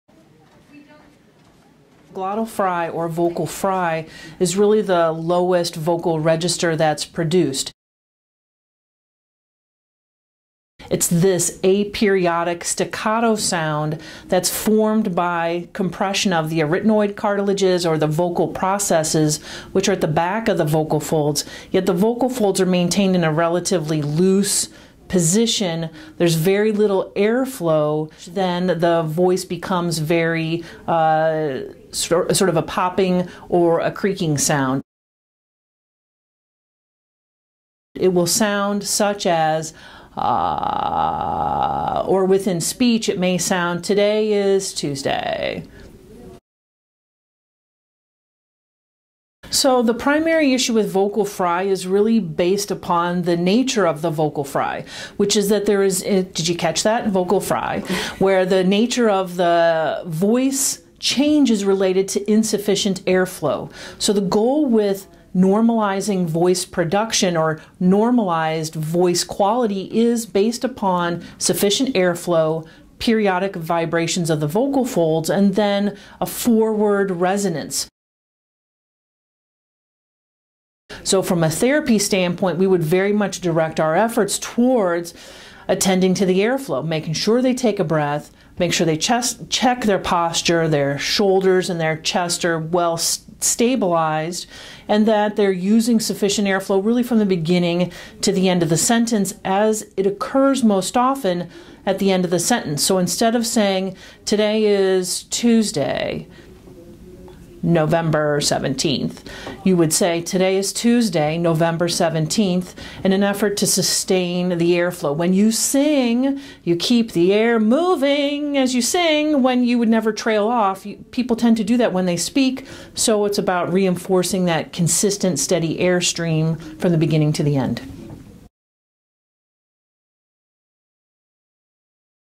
What Is Vocal Fry? A Speech-Language Pathologist Explains
The ASHA Leader asks Washington, D.C.-based SLP Laura Purcell Verdun four questions about vocal fry and how she treats it in clients.

TRANSCRIPT:
Glottal or vocal fry is really the lowest vocal register that's produced.

[What is Vocal Fry?]

It's this aperiodic staccato sound that's formed by compression of the arytenoid cartilages or the vocal processes which are at the back of the vocal folds. Yet the vocal folds are maintained in a relatively loose position - there's very little air flow - then the voice becomes very sort of a popping or a creaking sound.

[So What Does It Sound Like?]

It will sound such as, “ahhh,” or within speech it may sound [like], “Today is Tuesday.”

[How Do You Aim to Treat It?]

So the primary issue with vocal fry is really based upon the nature of the vocal fry which is that there is -- Did you catch that? “Vocal fry” -- where the nature of the voice changes related to insufficient air flow. So the goal with normalizing voice production or normalized voice quality is based upon sufficient air flow, periodic vibrations of the vocal folds, and then a four word resonance.

[How Do You Work On It With Clients?]

So from a therapy standpoint we would very much direct our efforts towards attending to the air flow, making sure they take a breath, make sure they check their posture -- their shoulders, and their chest are well-stabilized -- and that they're using sufficient air flow really from the beginning to the end of the sentence as it occurs most often at the end of the sentence. So instead of saying, “Today is Tuesday, November 17th.” You would say, “Today is Tuesday, November 17th,” in an effort to sustain the air flow. When you sing, you keep the air moving as you sing when you would never trail off. People tend to do that when they speak so it's about reinforcing that consistent steady air stream from the beginning to the end.